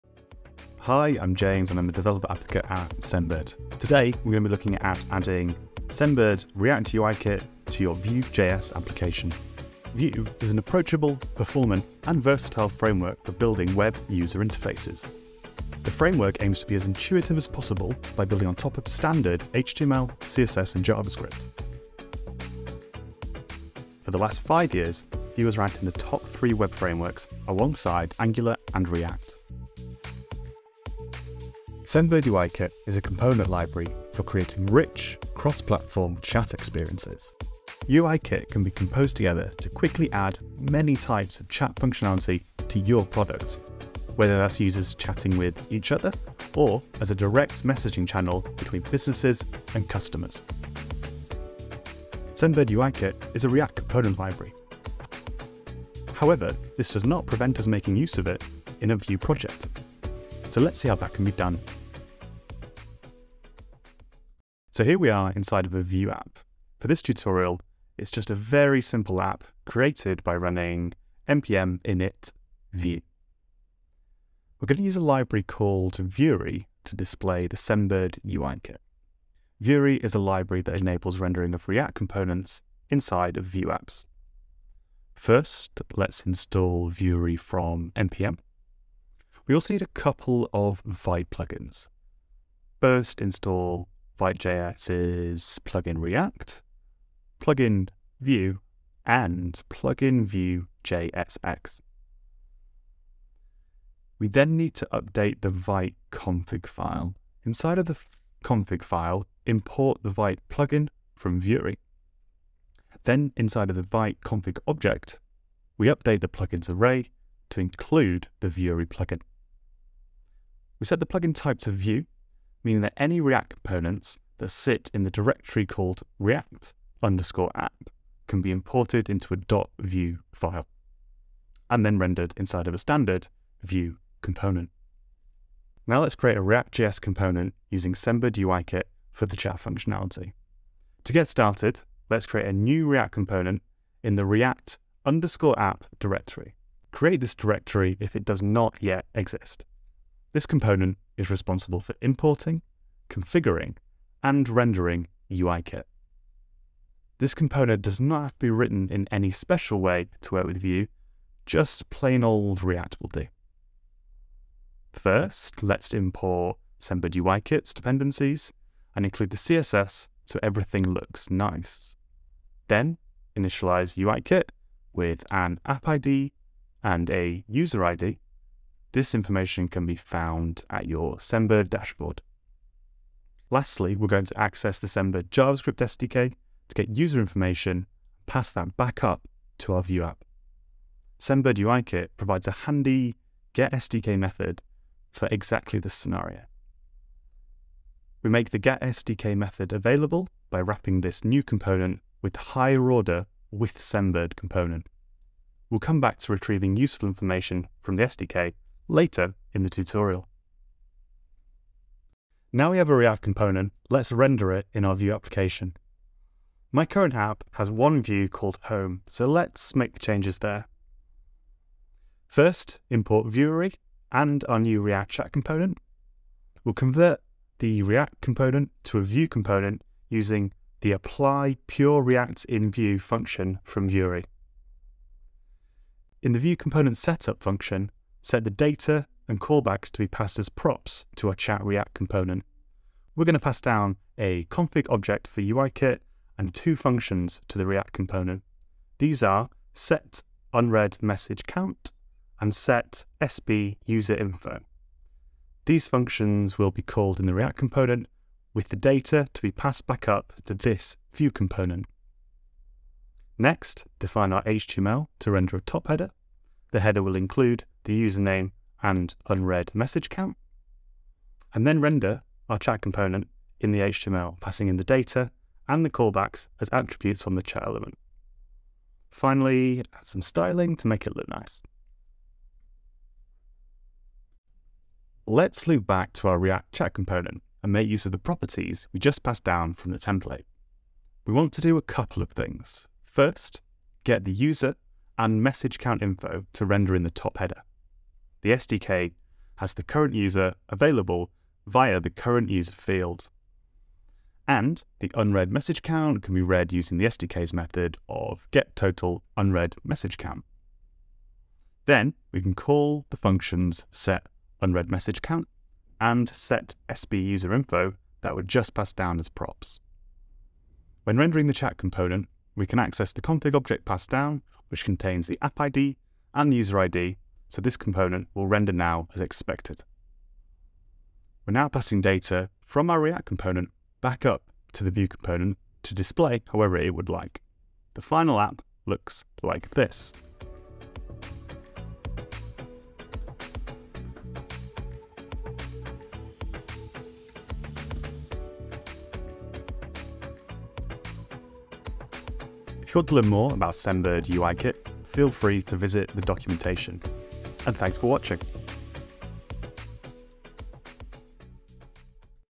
Build a Vue chat app using Sendbird’s UIKit | Render React components in a Vue chat app
Learn how to build a Vue chat app using the Sendbird UIKit. In this tutorial, we will learn how to use Sendbird UIKit in a Vue.js chat app. By leveraging the power of a library called Vueary, we will bridge the Vue and React worlds.

0:00 Intro
0:09 Intro to Vue
0:31 Intro to Sendbird UIKit
1:04 Installation & setup
2:15: Create a React.js component
5:39: Final app
5:52: Outro

▬▬▬▬▬▬  Join our free Developer plan!  ▬▬▬▬▬▬